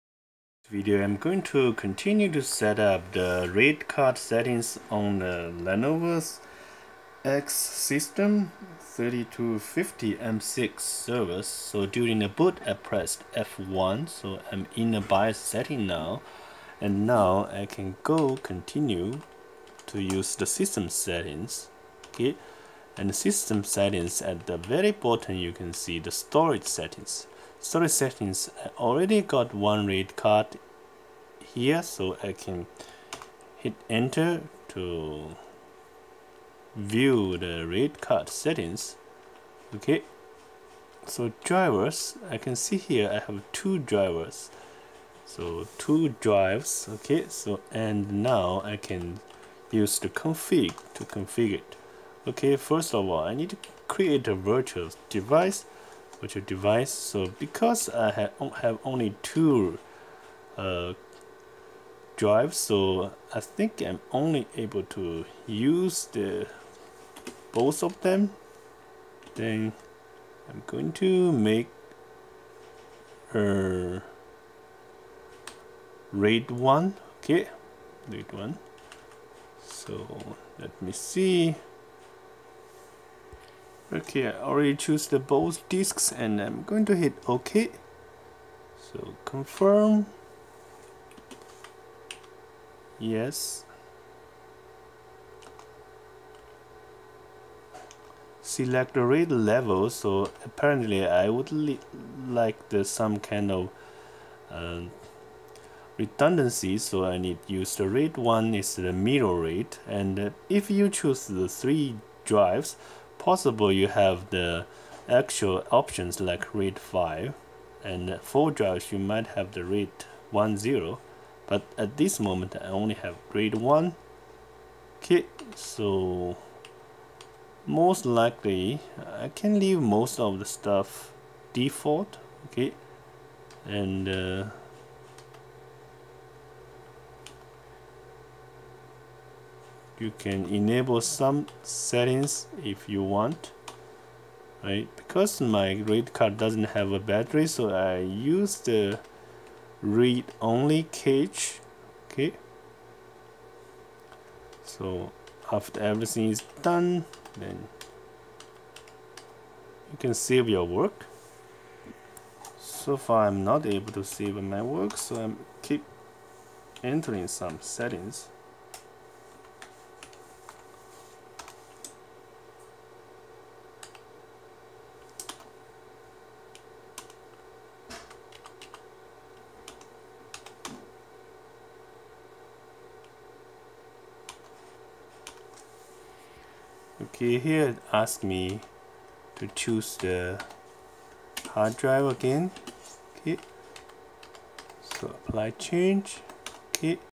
How to configure raid 1 or 5 on Lenovo X3250 Server
Note: In the video, I select Access Policy as Read-Only. This should not be the case in most systems. You do need set it as read/write in order to make it writable to your OS.